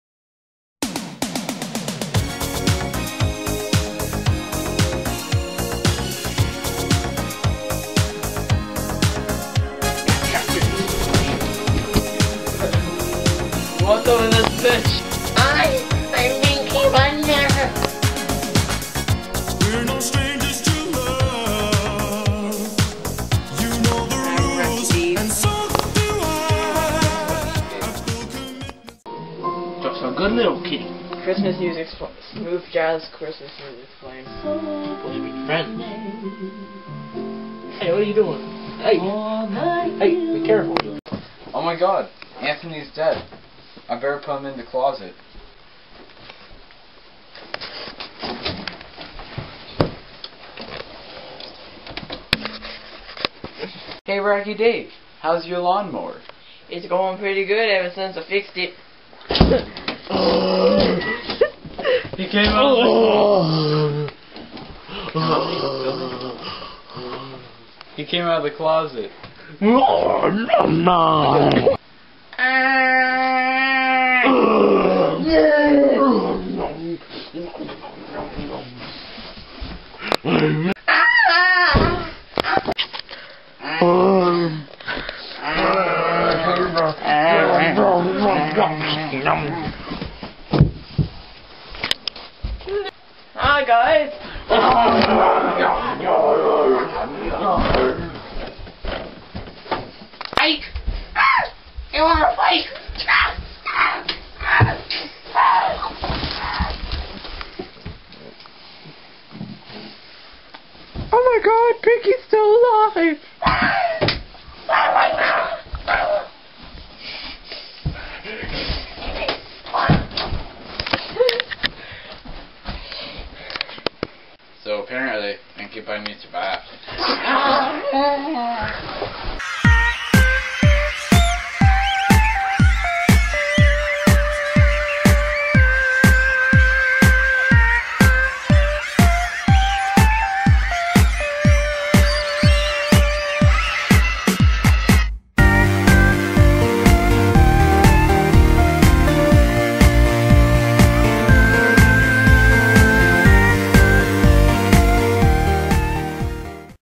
Not Zombieland
In a room with a zombie cat, all hell can break loose.

With Anthony Leroy in his first video with us.